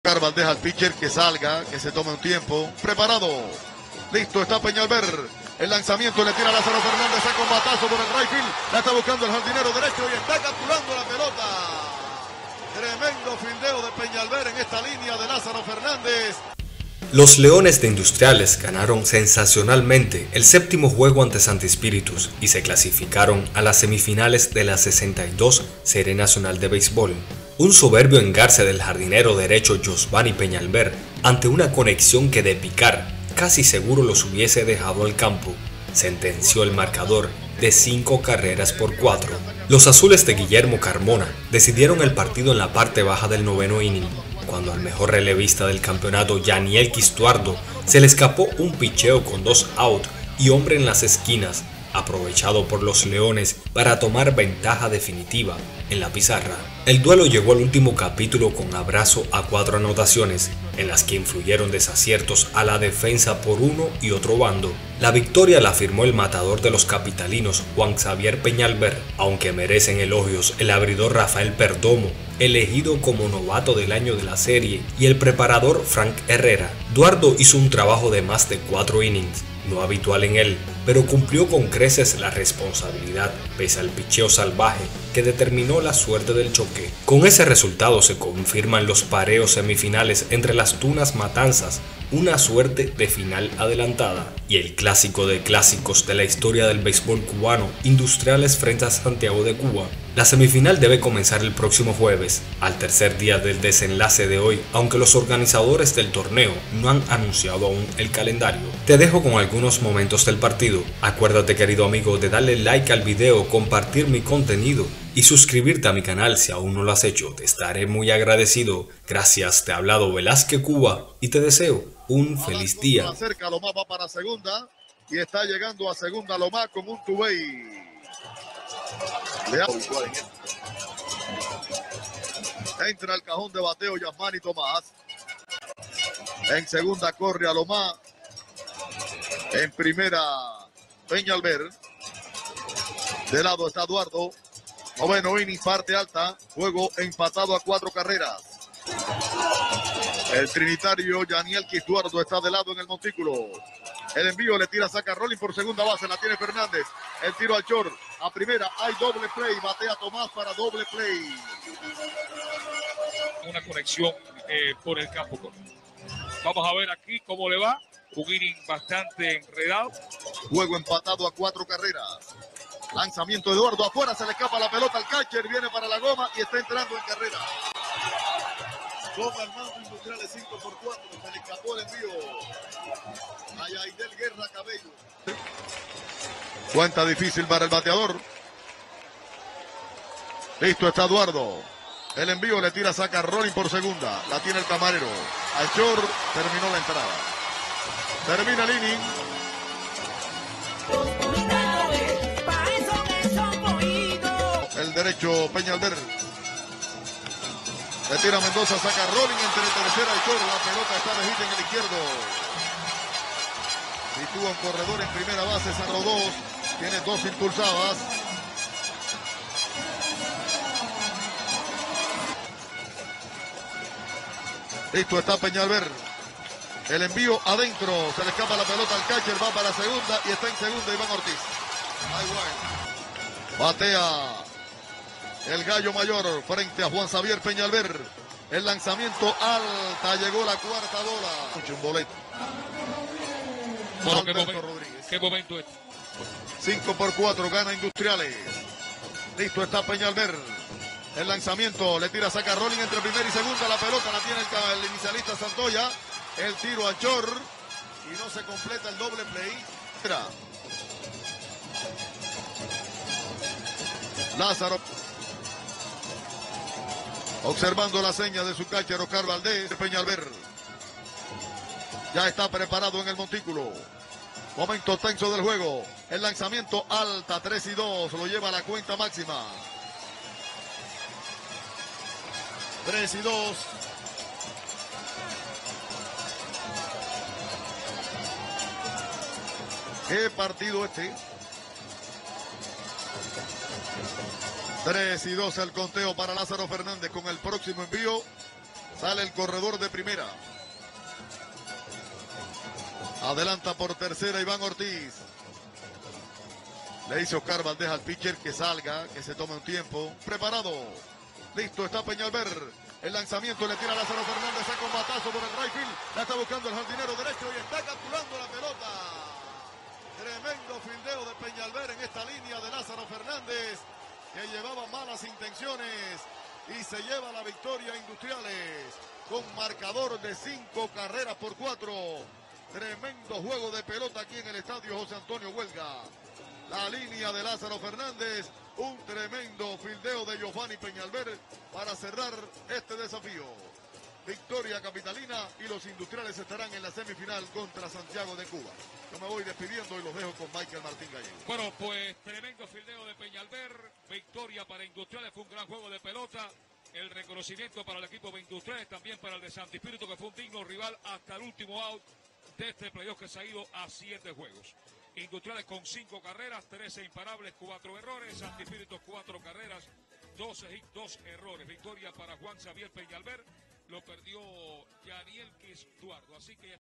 Industriales gana SENSACIONALMENTE en PLAYOFF del béisbol cubano
Un soberbio engarce del jardinero derecho Yosvani Peñalver ante una conexión que de picar casi seguro los hubiese dejado al campo sentenció el marcador de 5-4.

Los Azules de Guillermo Carmona decidieron el partido en la parte baja del noveno inning cuando al mejor relevista del campeonato, Yanielquis Duardo, se le escapó un picheo con dos outs y hombre en las esquinas aprovechado por los Leones para tomar ventaja definitiva en la pizarra.

El duelo llegó al último capítulo con abrazo a cuatro anotaciones, en las que influyeron desaciertos a la defensa por uno y otro bando.

La victoria la firmó el matador de los capitalinos, Juan Xavier Peñalver (2.0 IP, 1H, 3K y 1BB), aunque merecen elogios el abridor Rafael Perdomo, elegido Novato del Año de la serie, y el preparador Frank Herrera.

Duardo hizo un trabajo de más de cuatro innings, no habitual en él, pero cumplió con creces la responsabilidad, pese al picheo salvaje que determinó la suerte del choque.

Con ese resultado se confirman los pareos semifinales entre Las Tunas-Matanzas, una suerte de final adelantada, y el clásico de clásicos de la historia del béisbol cubano: Industriales-Santiago de Cuba.

La semifinal comenzará el próximo jueves, pero los organizadores del torneo no han anunciado aún el calendario.